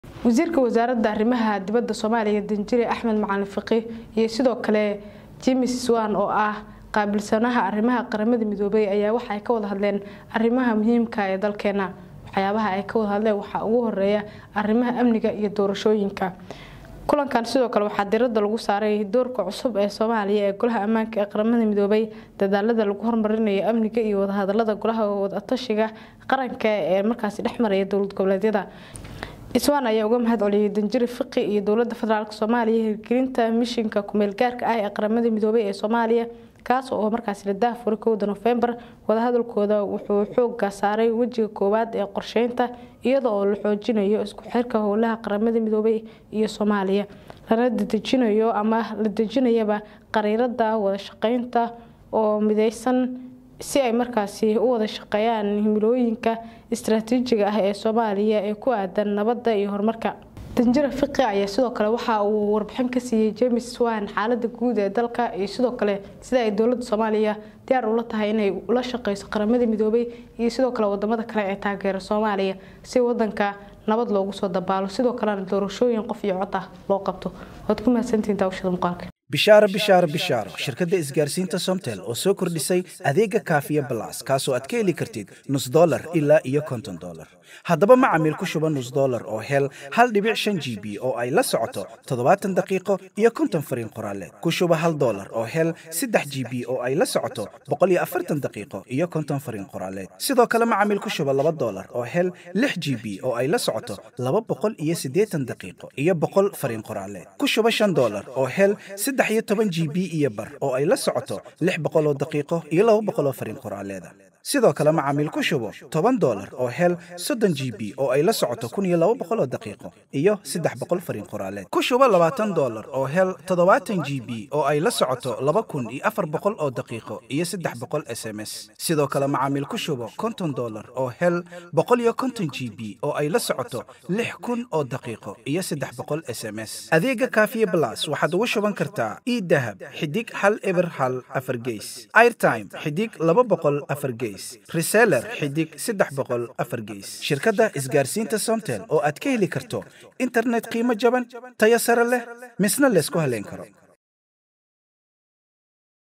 Maxeey ka wada hadleen Wasiir Fiqi iyo James Swan.?
Wasiirka Arrimaha Dibadda oo la kulmay James Swan si loo xoojiyo wada shaqeynta Soomaaliya iyo Qaramada Midoobay.